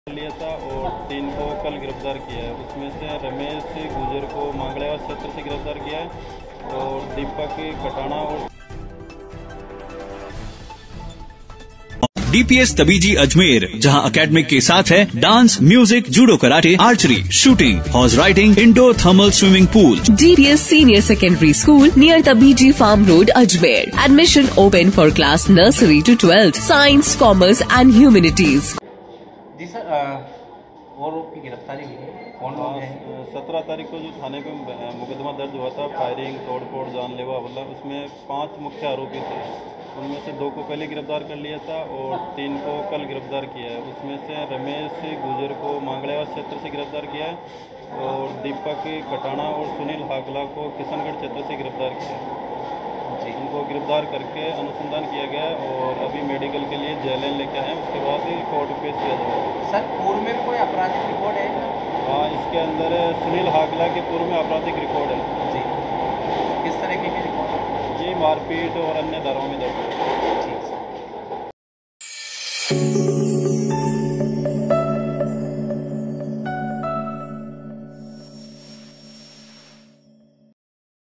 एनएसयूआई इकाई अध्यक्ष पर फायरिंग मामले में तीन भाई हुए गिरफ्तार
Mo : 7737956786